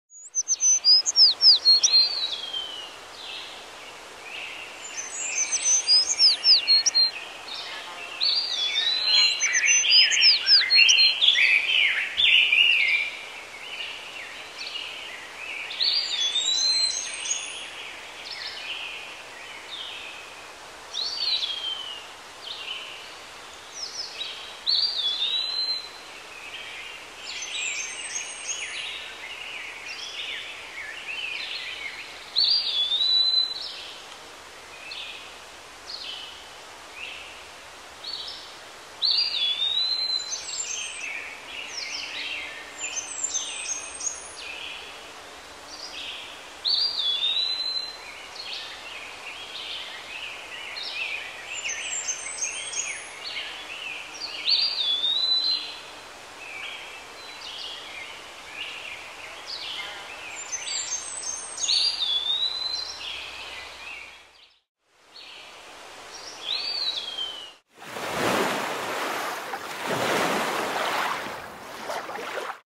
1406 Fallen Leaf Lake 2
This Monday we met at Fallen Leaf Lake. I had a nice view of Cathedral Peak and this wonderful blue boat.